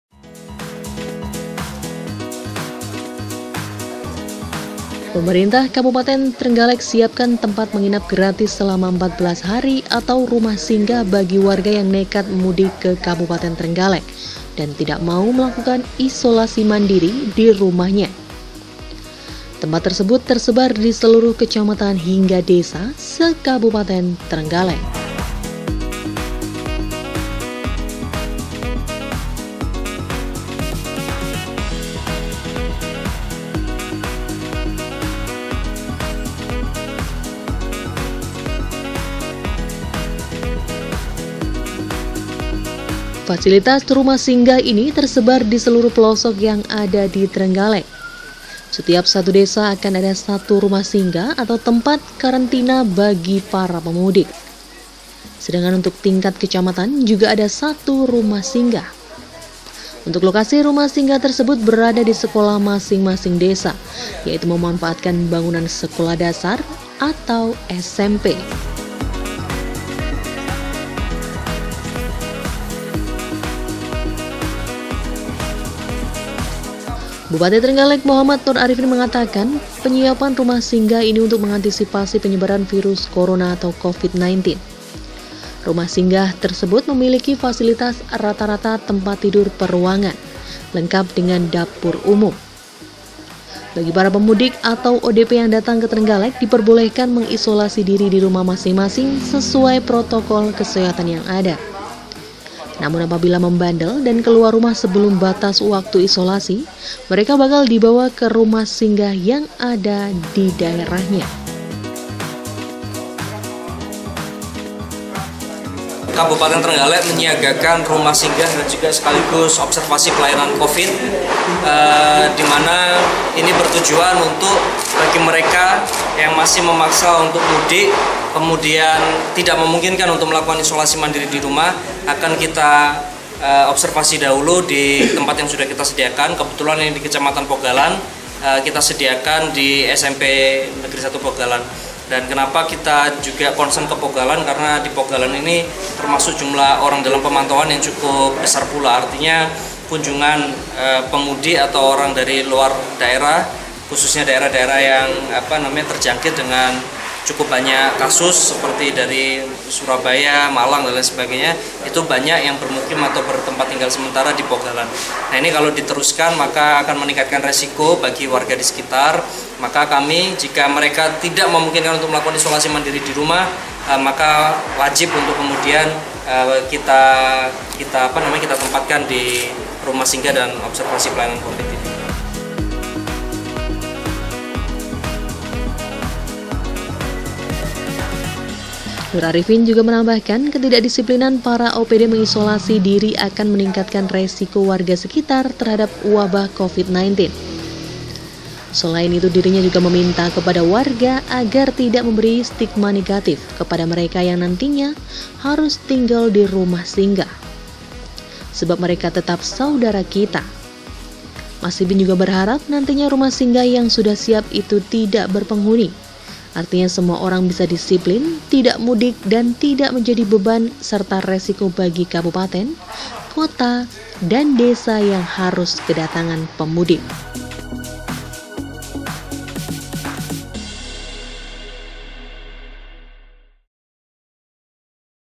Ngeyel Mudik & Tak Mau Isolasi Mandiri, Bupati Trenggalek Siapkan Pemudik Kamar Gratis Di Sekolah
- Pemerintah Kabupaten Trenggalek siapkan tempat menginap gratis selama 14 hari atau rumah singgah bagi warga yang nekat mudik ke Kabupaten Trenggalek, dan tidak mau melakukan isolasi mandiri dirumah. Tempat tersebut tersebar diseluruh kecamatan hingga desa se Kabupaten Trenggalek.